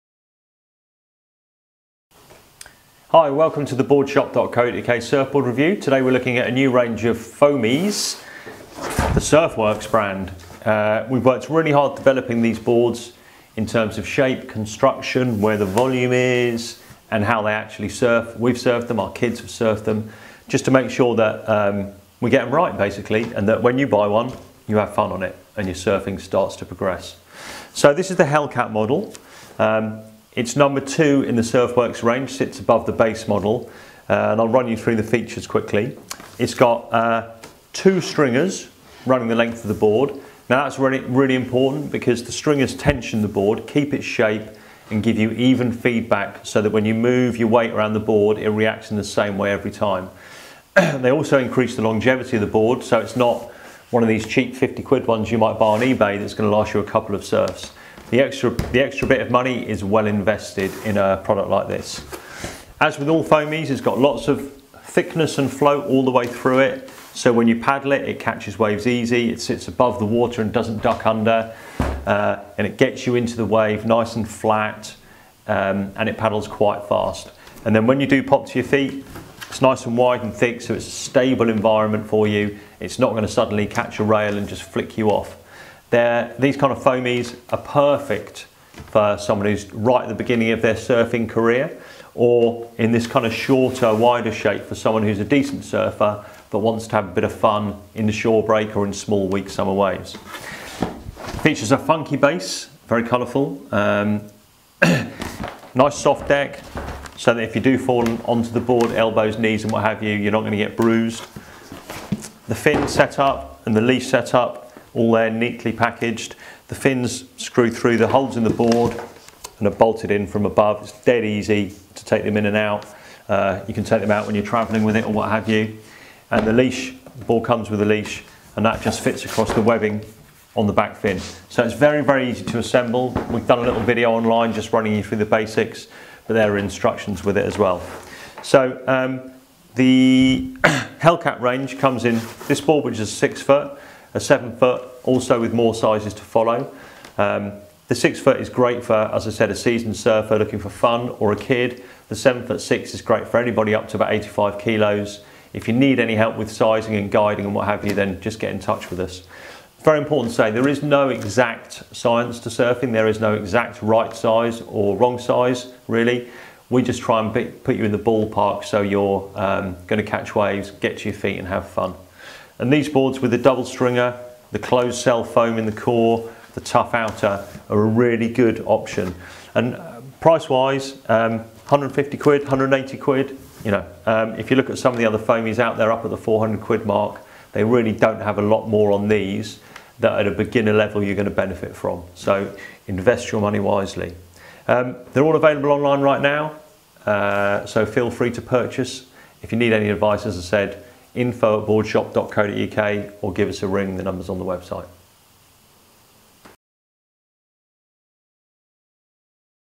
Surfworx Hellcat Mini Mal Surfboard Review
The Surfworx Hellcat Mini Mal soft surfboard is perfectly designed and constructed to get kids and light adults you up and riding on your very first surf session. All the key features are in place; nice wide and forgiving outline, soft padded deck to soften those inevitable knocks, ankle leash and a set of rubber safety fins to prevent injury to the rider and other surfers and swimmers. The Hellcat Mini Mal is constructed with a soft and forgiving foam deck and comes with a set of rubber safety fins to prevent injury to the rider, other surfers and swimmers.

The Hellcat Mini Mal is constructed with a High Density Closed Cell foam Core which means the boards are nice and lightweight, and should you be unlucky enough to damage the outer durable EBS deck skin the board won't soak up water, become waterlogged and un-surfable. The board has twin resin coated wooden stringers which helps the board maintain its shape and perform on the wave. The high density HDPE slick bottom is nice and durable and helps the board paddle fast and gives a smooth ride. The High Density deck skin is very durable and will keep the Hellcat Mini Mal performing for years to come.

The Surfworx Hellcat comes with a three fin "thruster" setup which gives great control and a nice stable and predictable ride.

This brilliant mini mal is a tried and tested shape which has proven to be the perfect platform and design to kick-off your surfing adventure. This smaller 6ft model is aimed at kids who are just starting out or intermediate, lightweight adults looking for a great value funboard for the summer.

The Boardshop team are super-excited about this Surfworx range of soft deck surfboards which has a board for anybody whether you're a kid new to surfing or an adult looking for a fun surfboard to improve your skills.